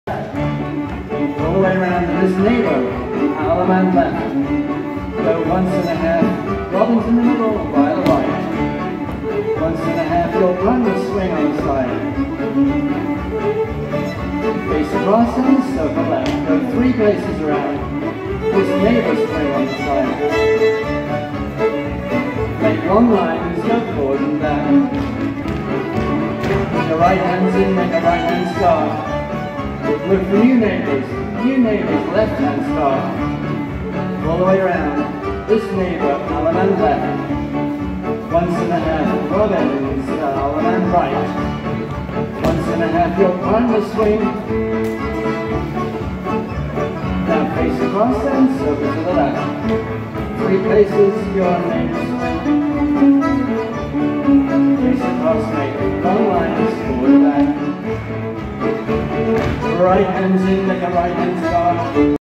FND 2024-08-09 The Orphans with Gwyn Williams calling! 20:31:45
At the Friday Night Contra Dance in the Spanish Ballroom at Glen Echo, Gwyn Williams calls to the music of The Orphans! Peter Buchak on accordion, Ben Kennedy on keyboard, and Miranda Weinberg on fiddle.